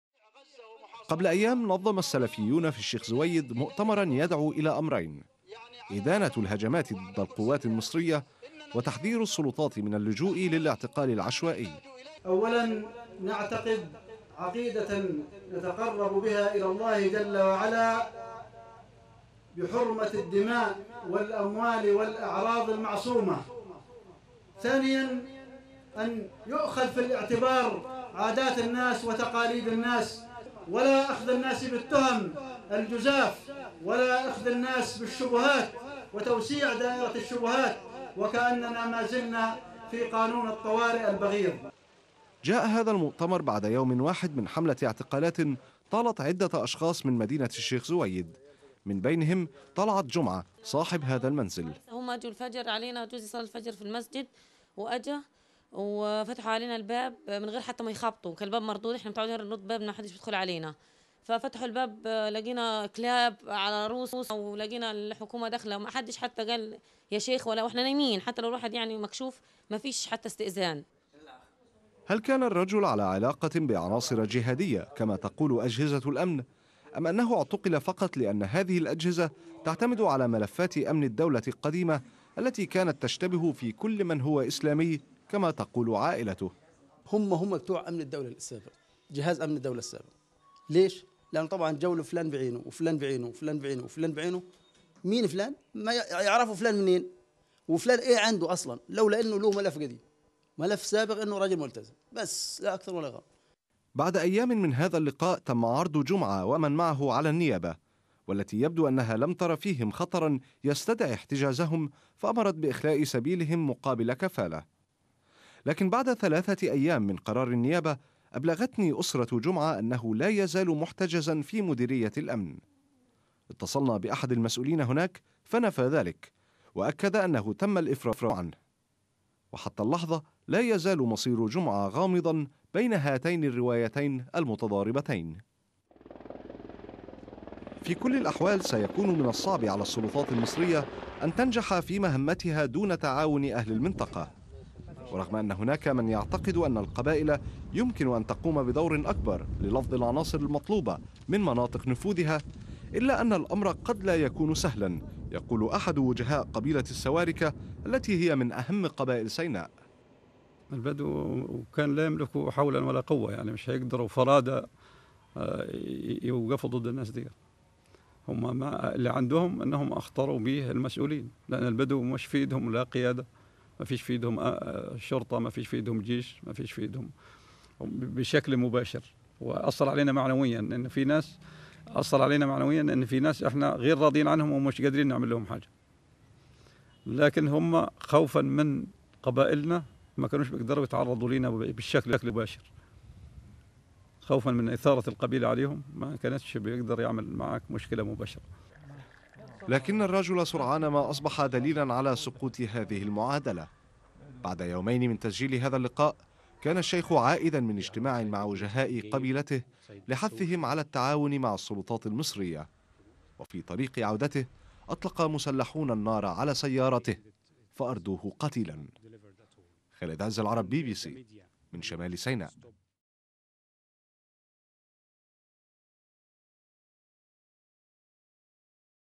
الجيش المصري يواصل العملية العسكرية في سيناء
مراسل بي بي سي  خالد عز العرب يقضى أسبوعا متابعا سير العمليات العسكرية في شمال سيناء.